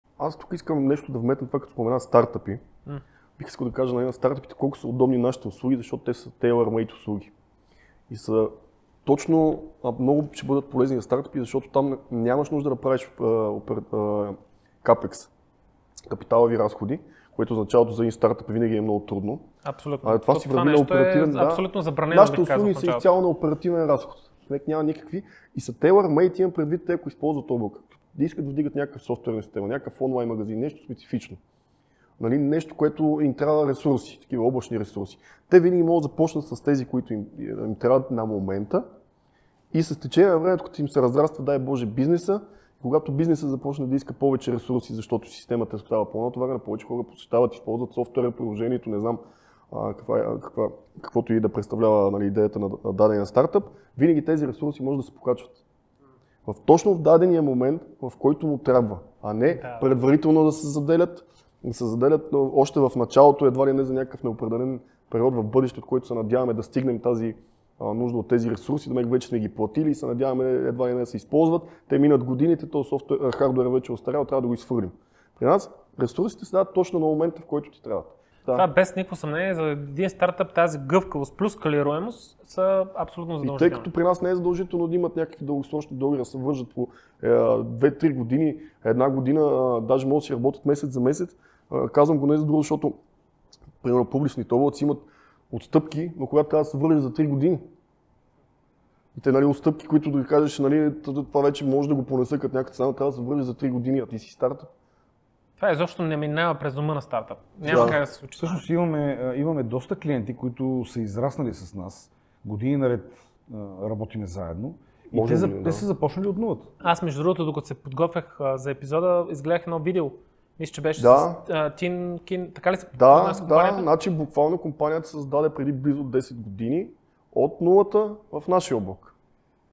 Старт БГ Подкаст C3 E09 Daticum | Услугите които са best fit for startups
В @Daticum  ИТ ресурсите са налични точно на момента, в който ти трябват. Това е задължително условие за стартъпа, който цели да постигане гъвкавост и скалируемост. Удобството на нашите услуги помага на стартъпите бързо валидиране на идеите им. Няма нужда да се правят капиталови разходи, което в началото винаги е много трудно. Нашите услуги са изцяло оперативен разход и са tailor-made. Т.е ако използват облачни услуги, винаги могат да започнат с ресурсите, които им трябват на момента. Когато бизнесът започне да иска повече ресурси, защото системата е станала по-натоварена, тези ресурси могат да се покачват точно в дадения момент, в който са необходими. Така няма нужда предварително да се заделят средства, което дава възможност на стартъпа да бъде оперативно гъвкав.